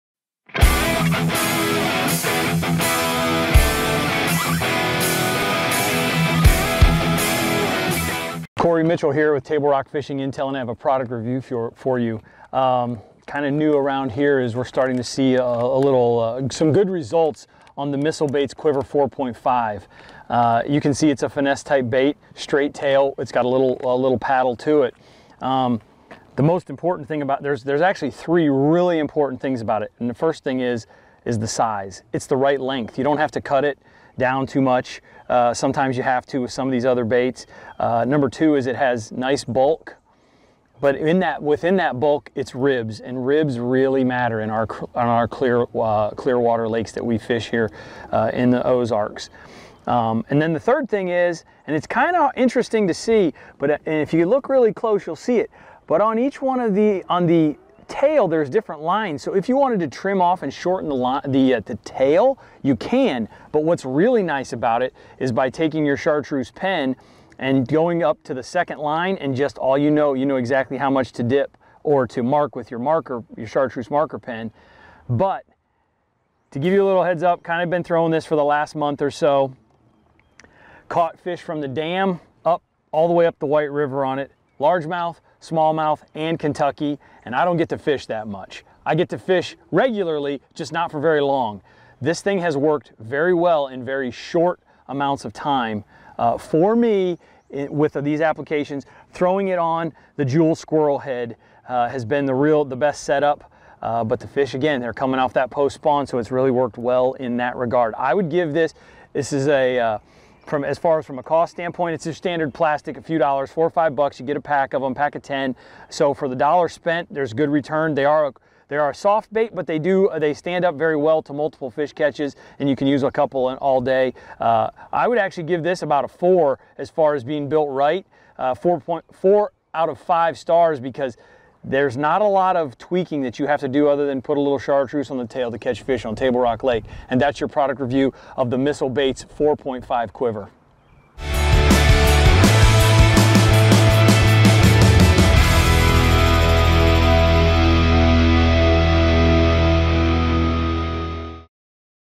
Product Review: Missile Baits Quiver 4.5
Table Rock Fishing Expert Kory Mitchell reviews the Missile Baits Quiver 4.5 Worm